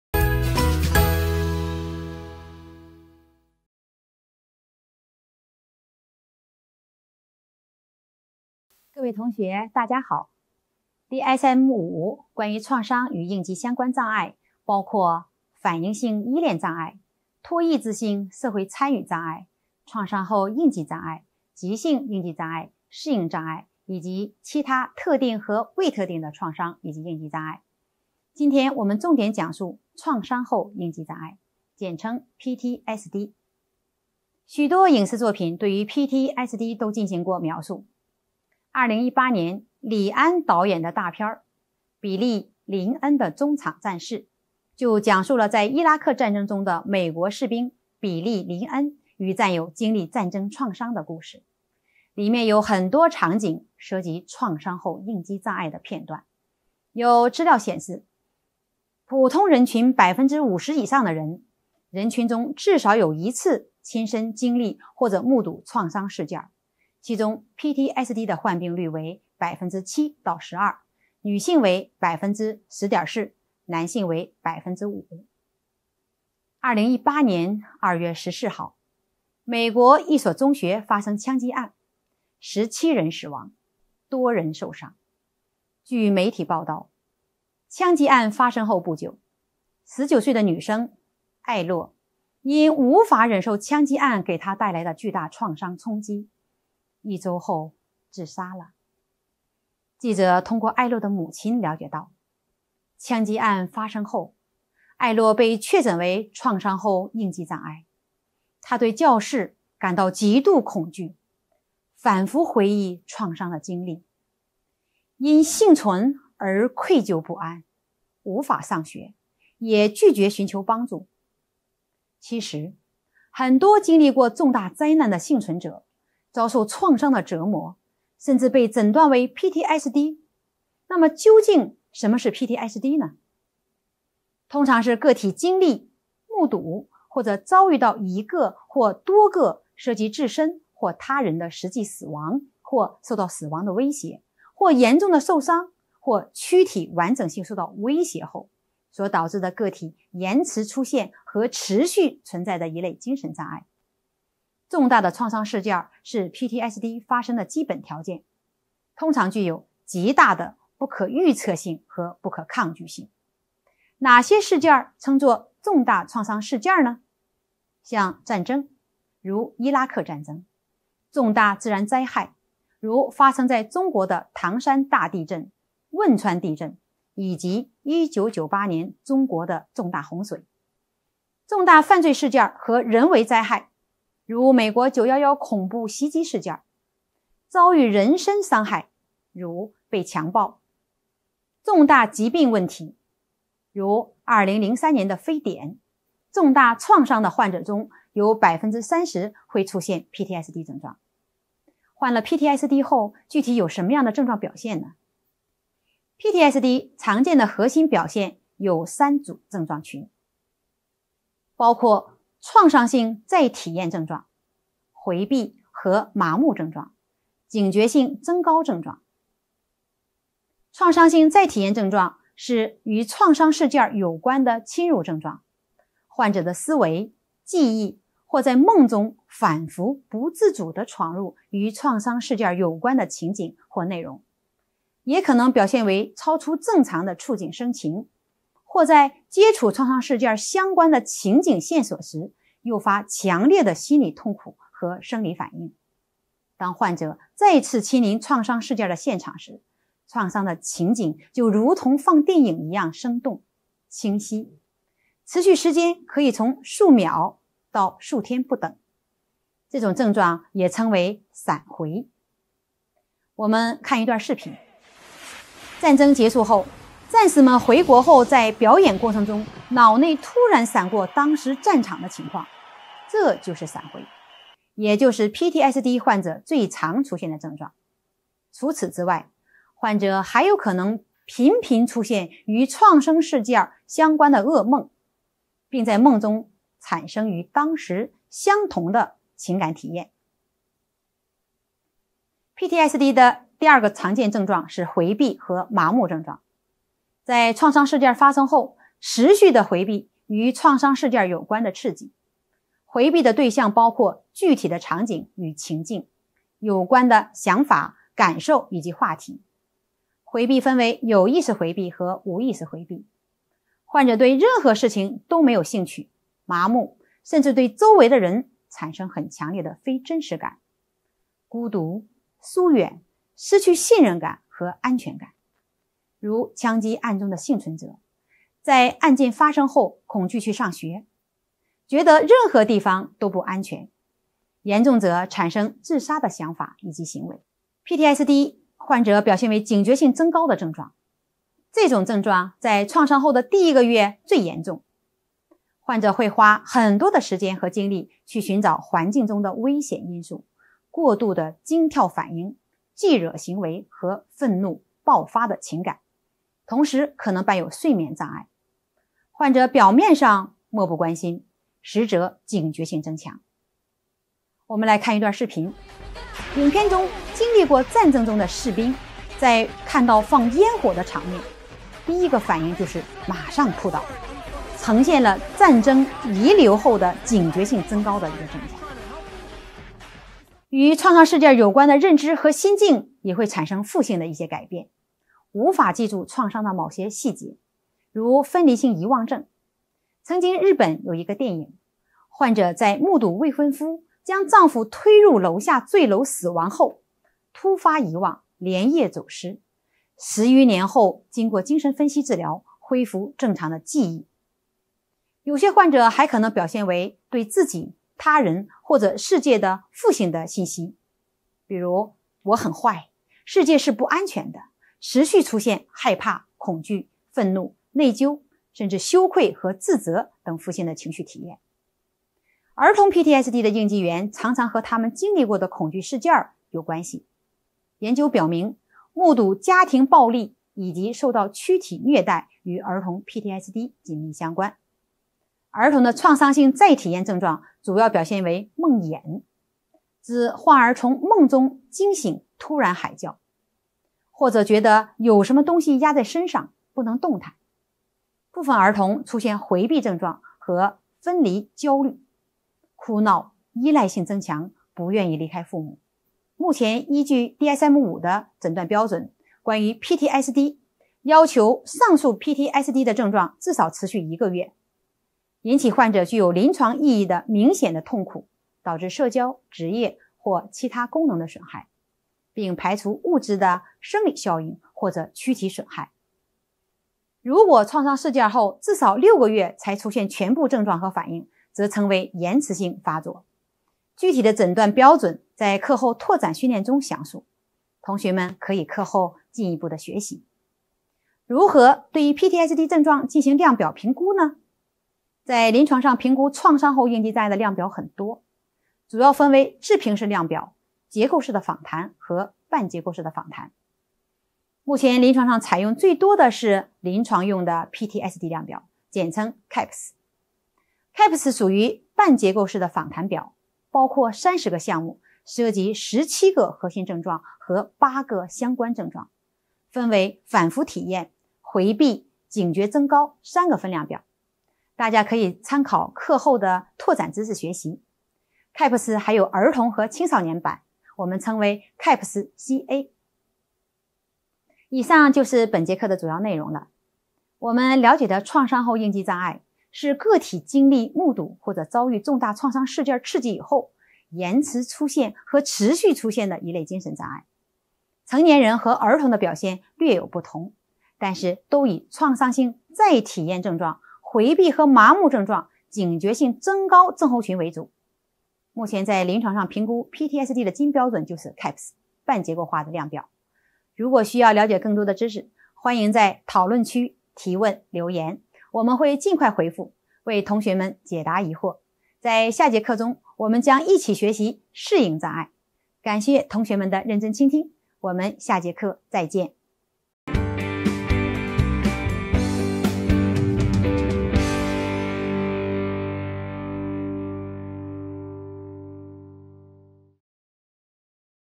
人格与精神障碍 应激障碍 创伤后应激障碍 EP 41
本课程基于全生命周期理念，了解人格与人格障碍的形成，从儿童发育问题、抑郁与双相障碍、精神分裂症、强迫及焦虑症、恐怖症、应激创伤、进食与失眠障碍，到老年痴呆等各类精神障碍的成因、临床症状、诊断评估、药物治疗与心理治疗。通过本课程，探索精神障碍背后的人格，掌握诊断与治疗原则。